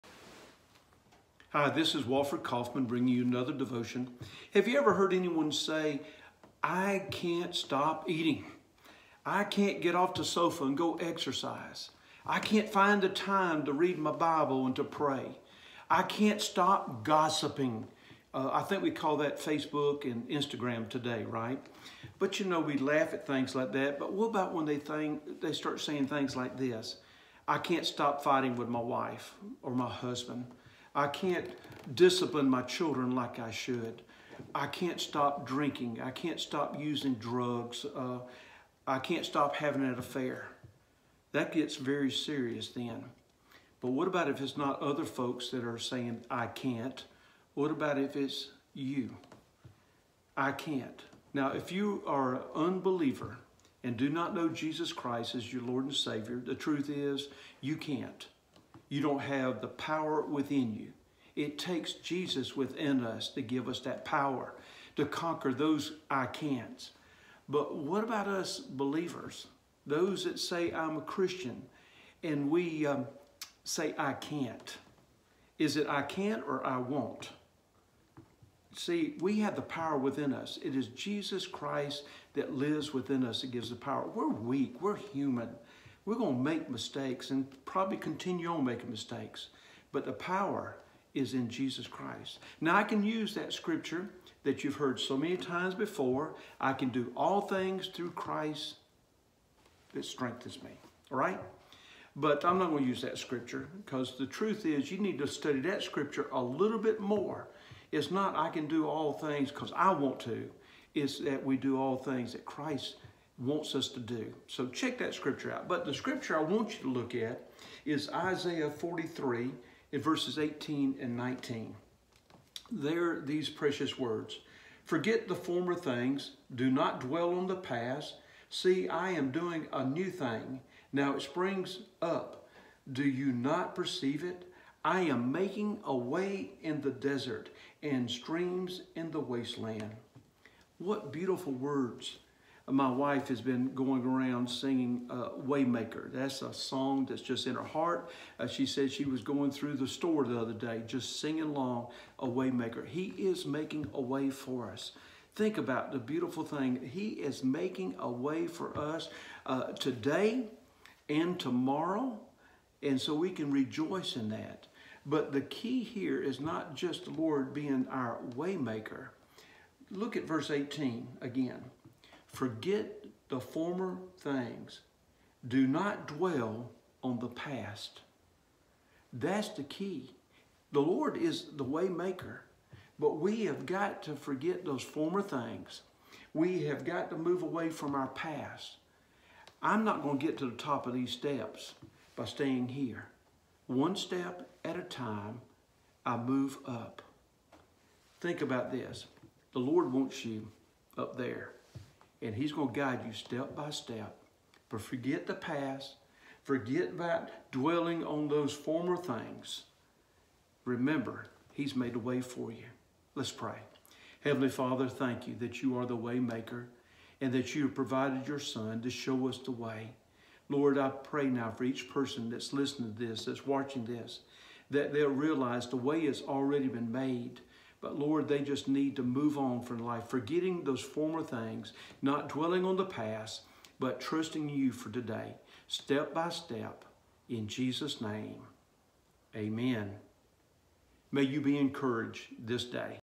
I Can’t
Have you said the words "I Can't" lately?  Truth is you can't! Devotion by Wofford Caughman based on Isaiah 43:18-19.

Thanks to fitnesstogether.com for the intro picture!